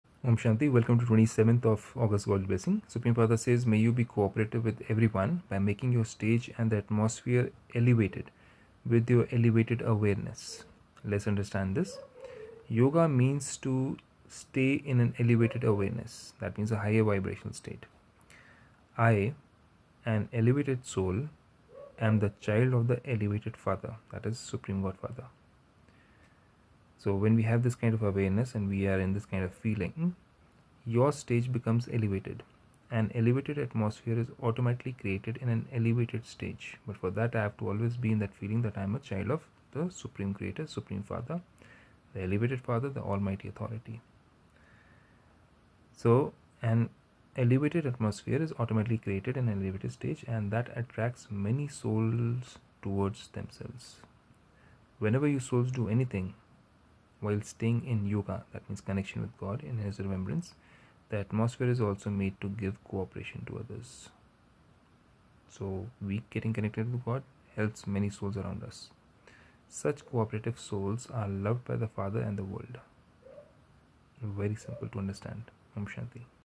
Be co-operative with everyone by making ur stage & atmosphere elevated with ur elevated awareness
DAY 946 May you be co-operative with everyone by making your stage and the atmosphere elevated with your elevated awareness